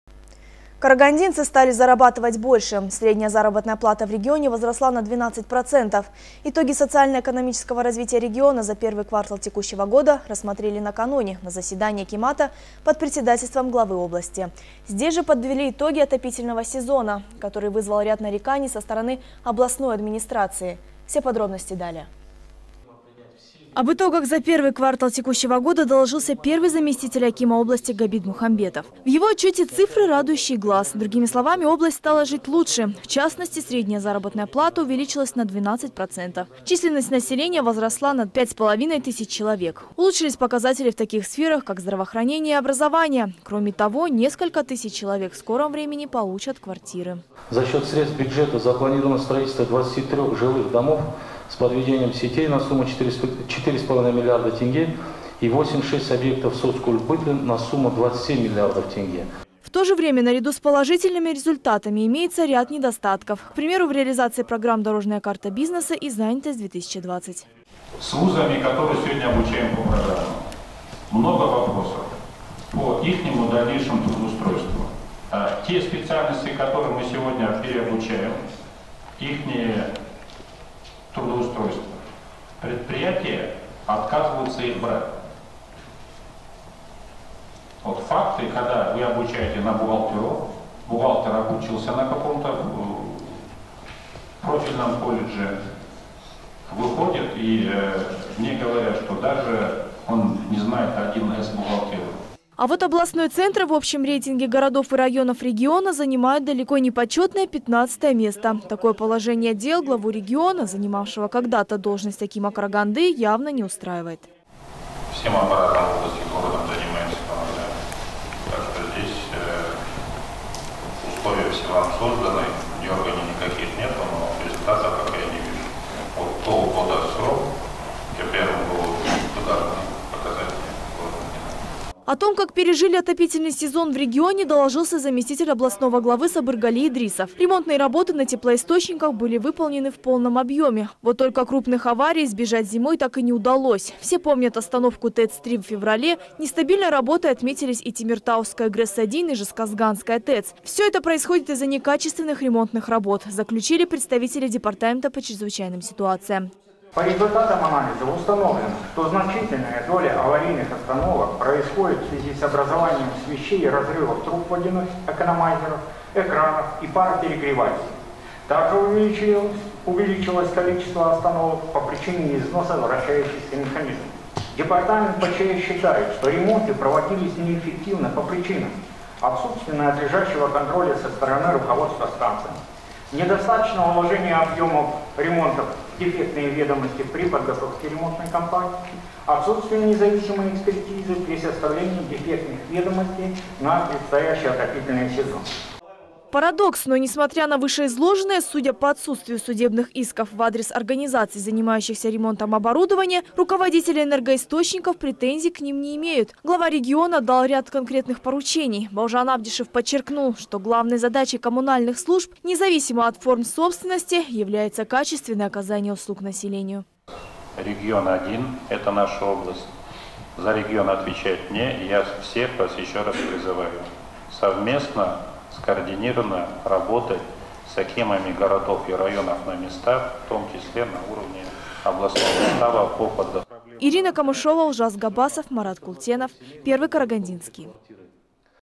Карагандинцы стали зарабатывать больше
Средняя заработная плата по региону возросла на 12 %. Итоги социально-экономического развития региона рассмотрели накануне на заседании акимата под председательством акима области. Здесь же подвели итоги отопительного сезона, которые вызвали ряд нареканий со стороны областной администрации.